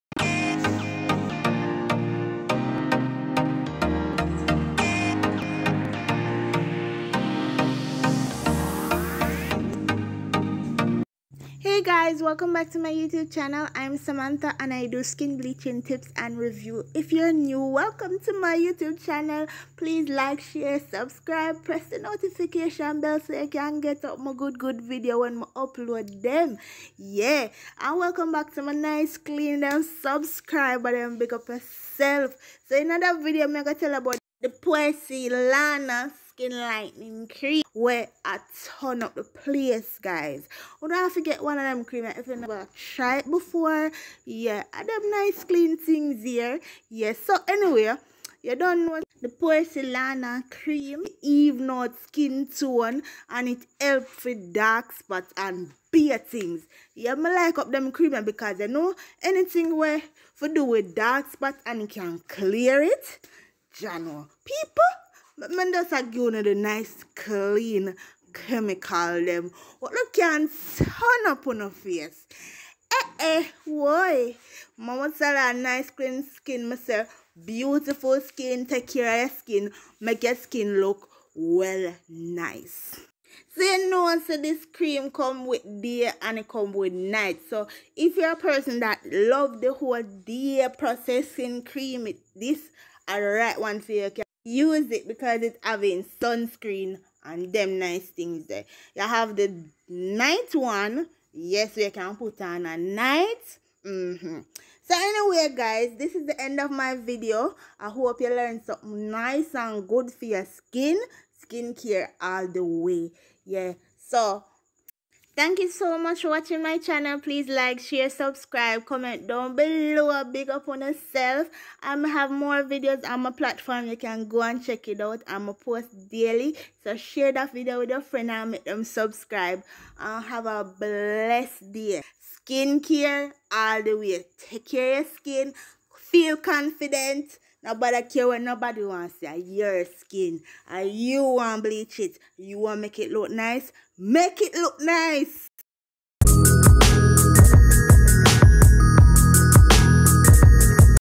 Porcelana Skin Bleaching /Lightening Cream For Dark Spot| Even Skin Tone | Product Review
Porcelana Daytime & Nighttime Skin Bleaching /Lightening Cream For Dark Spot and Even Skin Tone Fast

If you are looking for a cream to lightening you dark spot you can use this cream.

Time Stamp
Start 0:00
Ends

App
Imovie
Thumbnail